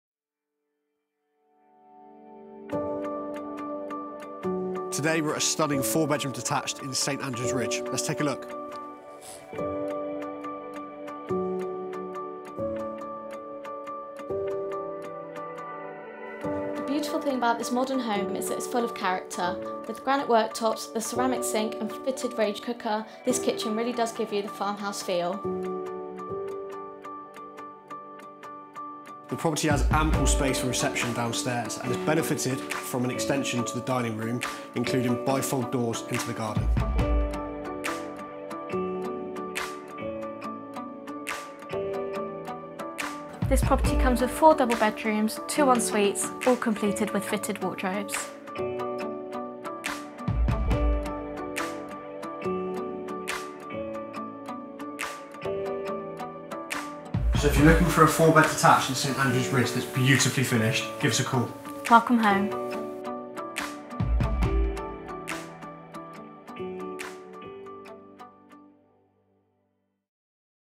Northbourne Road | North Swindon
Allow Josh and Beau, from our North Swindon Team, to show you around this beautiful 4-bedroom home in St Andrews Ridge.